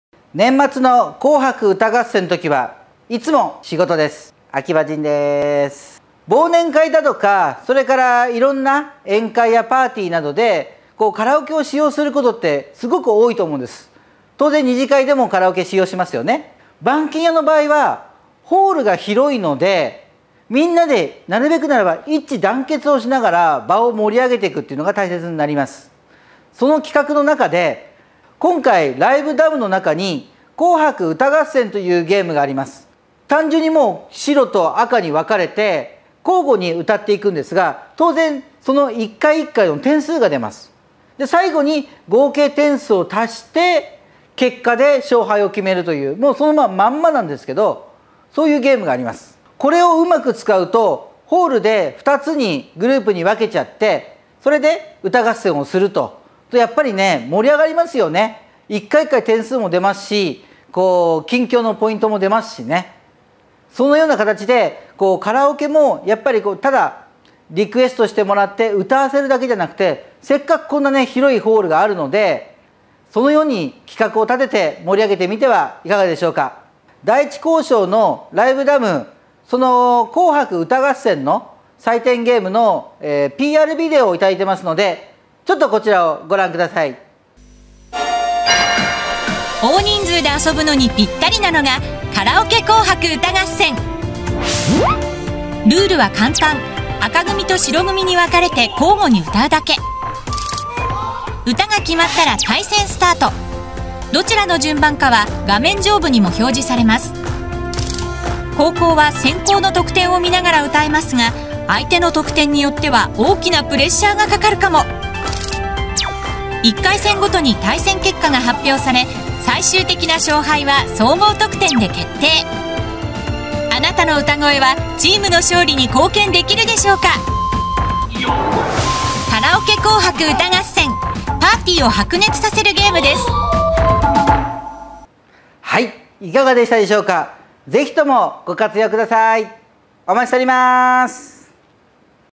カラオケ紅白歌合戦で忘年会やパーティーを盛上げよう！秋葉原
秋葉原バンキンヤのカラオケが新しくなり、コンテンツも充実しています。そんなカラオケを使い、忘年会やパーティーを盛上げてみませんか？カラオケは第一興商の最新機種 ライブダム（LIVE DAM）です。その中に「カラオケ紅白歌合戦」というゲームがあります。
年末にテレビで放送されるのとまったく同じルールです。一味違ったパーティーを企画してみませんか？ぜひともご相談お待ちしております。
英語字幕は画面右下のキャプションボタンを押してください。

The karaoke of Akihabara "Bankinya" became new. Do you use karaoke and enliven neither a year-end party nor a party? Karaoke is newest model "LIVE DAM" of "Daiichikosho." A "karaoke New Year's Eve song show" game is in the contents of karaoke.
It is the completely same rule as being broadcast in a program of Japan at an end of the year. Don't you plan the just a little bit different party?
Please use karaoke.

The English caption should push a caption button.

秋葉原バンキンヤ詳細